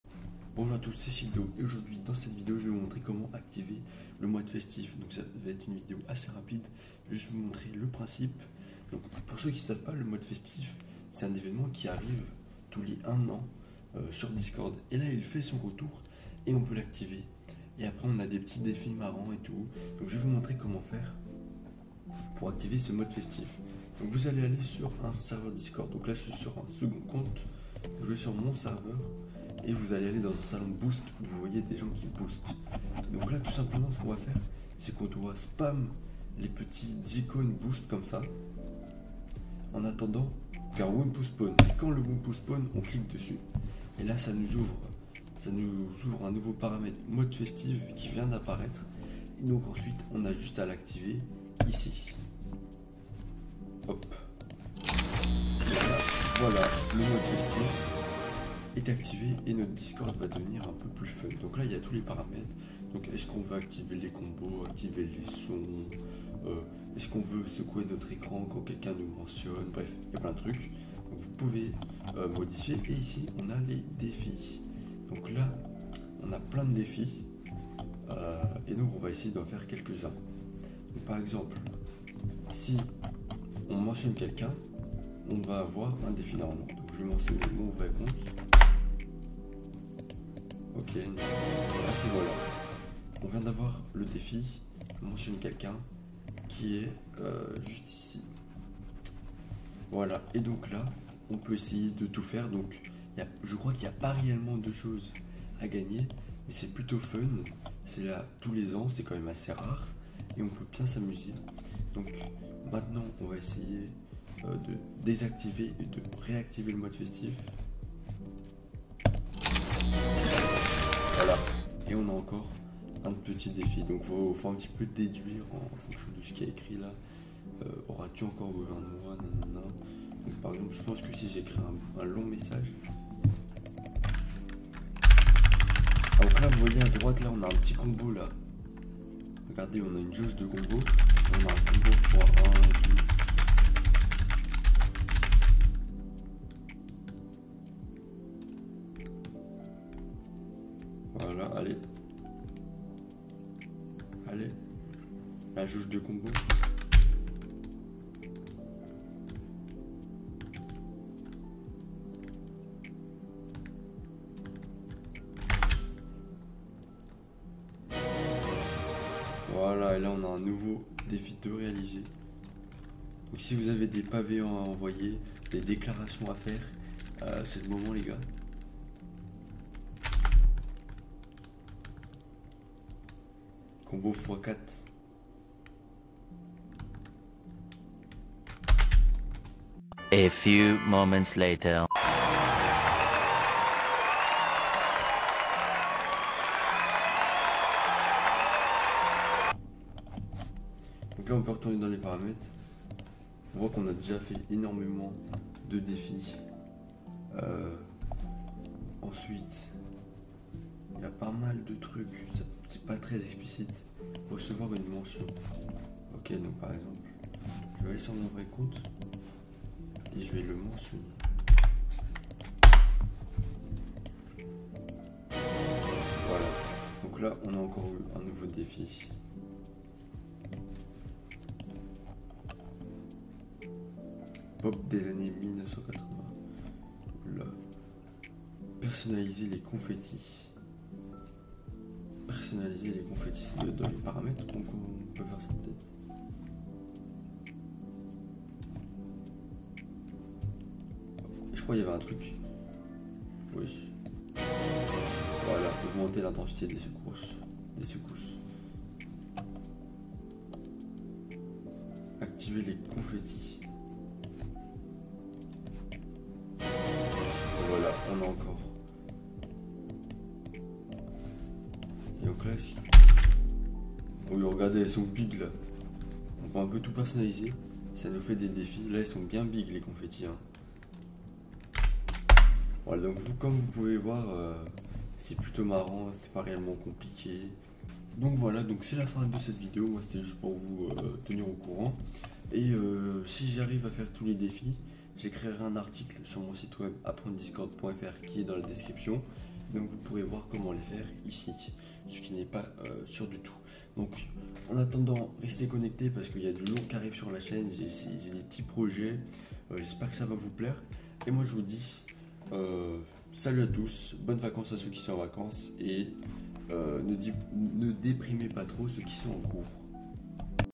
Le nouveau MODE FESTIF de Discord est incroyable !! 🎉
Dans cette vidéo, je vous montre le tout nouveau mode festif de Discord disponible pour une durée limitée. Je trouve ça plutôt marrant, et j'espère que vous allez réussir les défis de cette année !